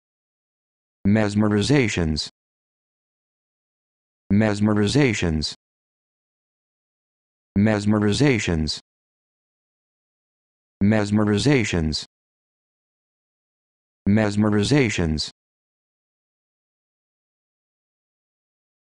How to Pronounce Mesmerisations
This video shows you how to pronounce Mesmerisations